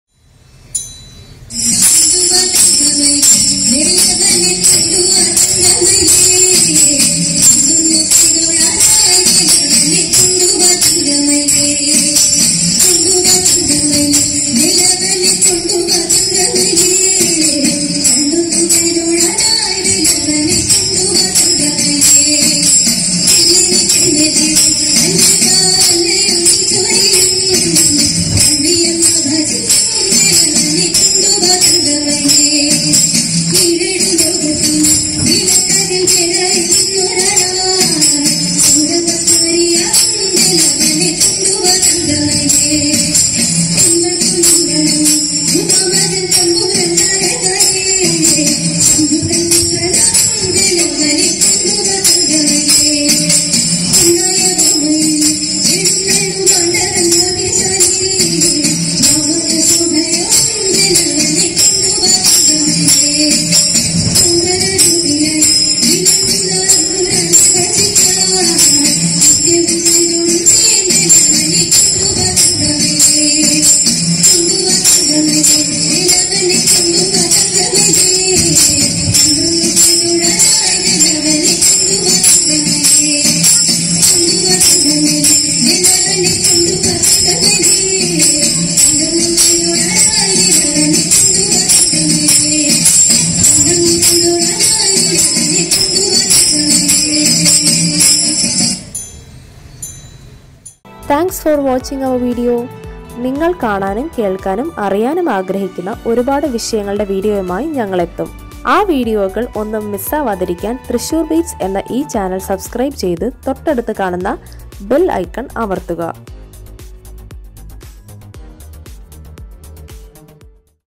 Srisailam Tiruvathirakali SangamViyyur | AATHIROLSAVAM SRI VADAKKUNNATHAN TEMPLE | THIRUVATHIRAKA LI
Srisailam Tiruvathirakali Sangam, Viyyur

Groups of up to eight or ten women are seen dressed in traditional Kerala attire, dancing in a circle. The graceful movements of the dance are both enchanting and elegant. The white saris with colourful borders along with fresh jasmines adorning their hair make for a wonderful sight. Fidelity and the power of female energy are the crux of this ancient practice.

Thiruvathirakkali
Kaikottikali
Traditional Kerala Dance Form
Performed During വടക്കുനാഥന്‍ ആതിരോത്സവം മണ്ഡപം
Sree Vadakkumnathan Temple
Palukassery Thiruvathira Team Viyyur